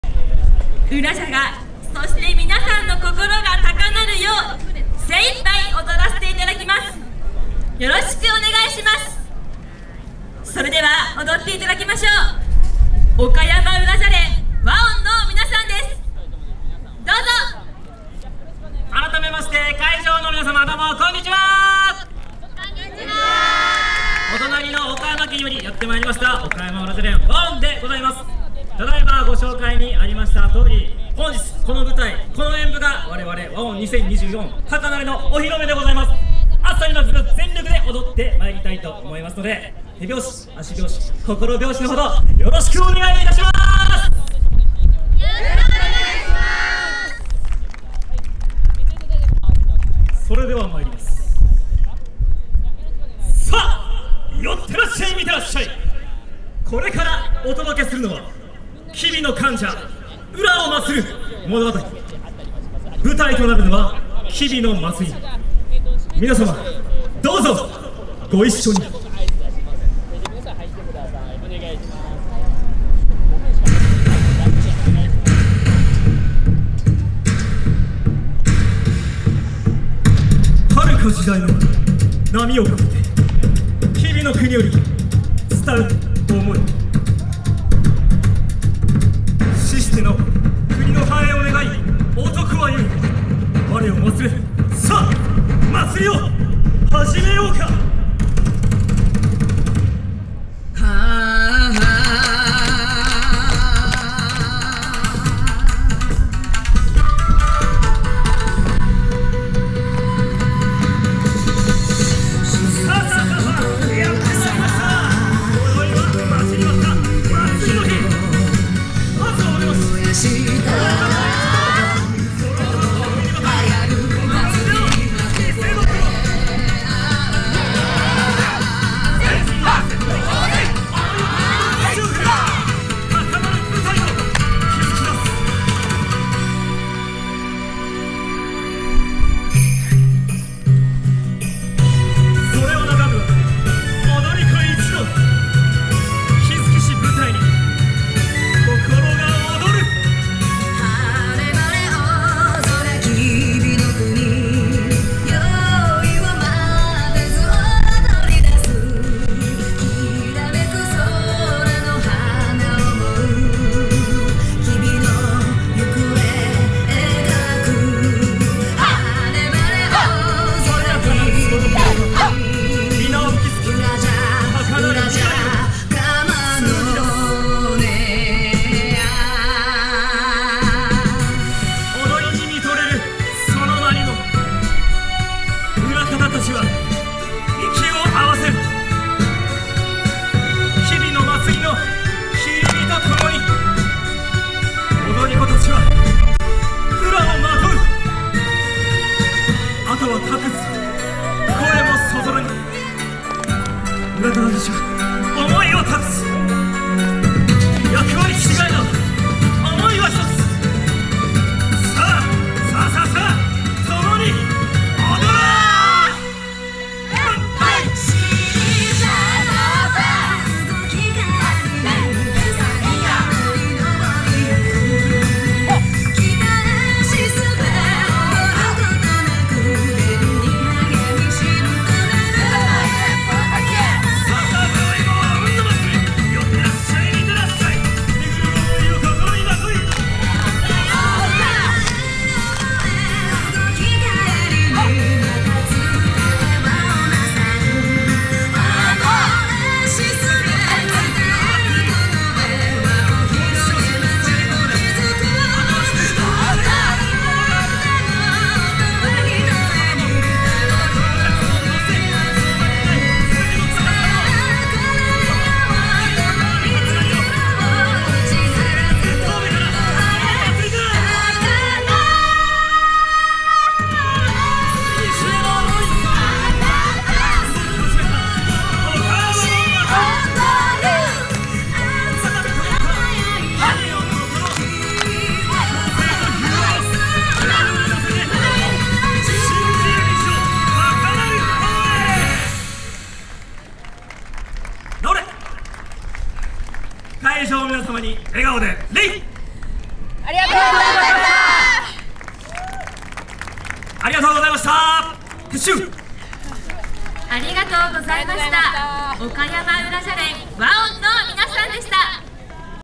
[4K] 岡山うらじゃ連 環温 Sailing KOBE 2024 サブ会場
Sailing KOBE 2024
2024年7月6日(土)
メリケンパーク
サブ会場
※公式カメラマンとして撮影